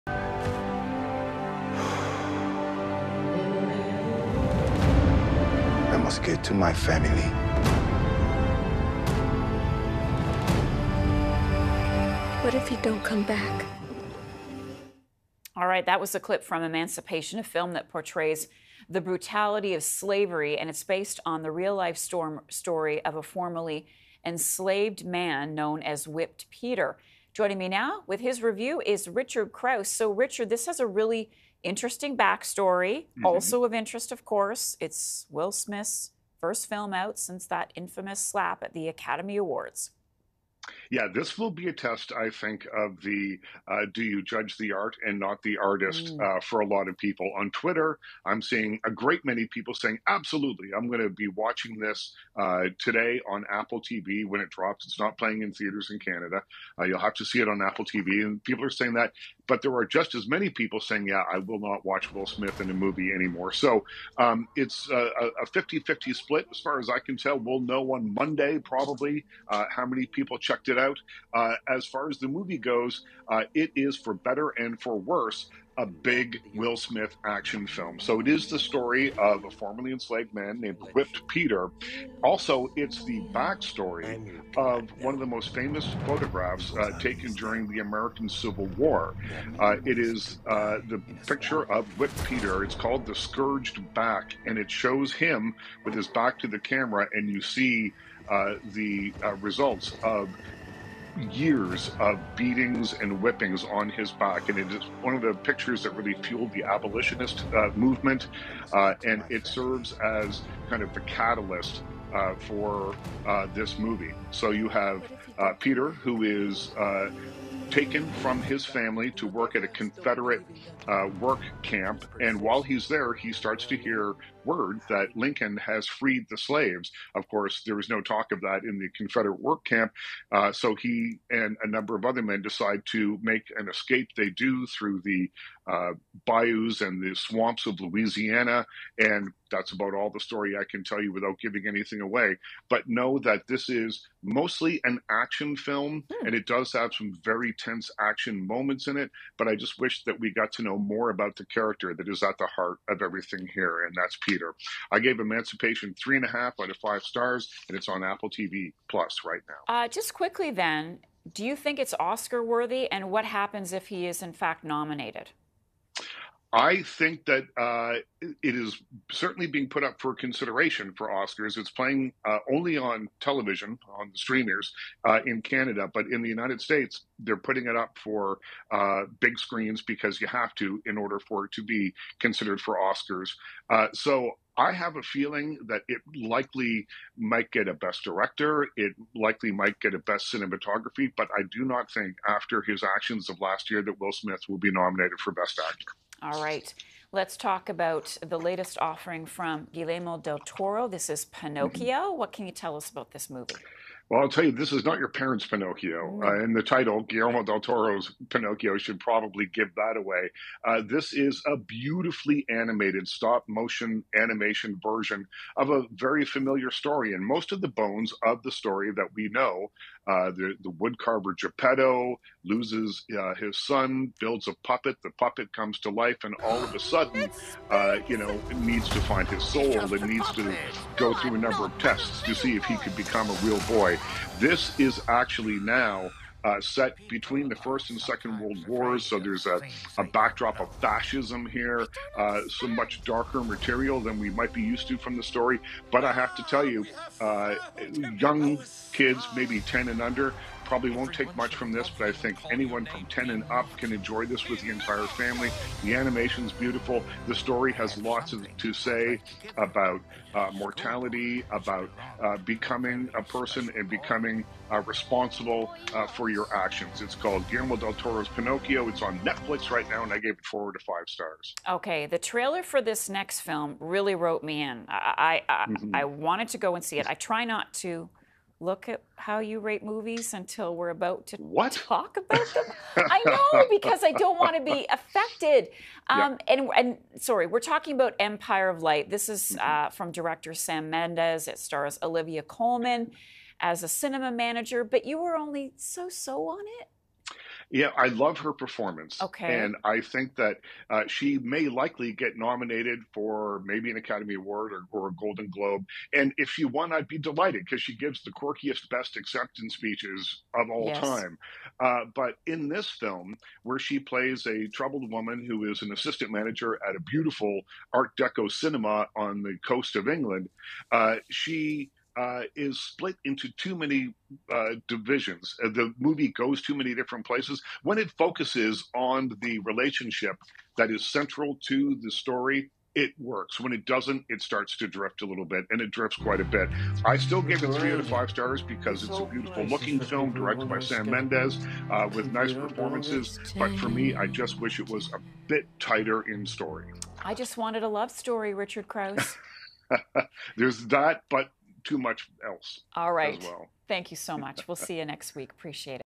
Will Smith's new movie | 'Emancipation a big Will Smith action film'
Film critic Richard Crouse shares his take on newly released movies, including 'Emancipation' and why he thinks it will likely receive an Oscar nomination.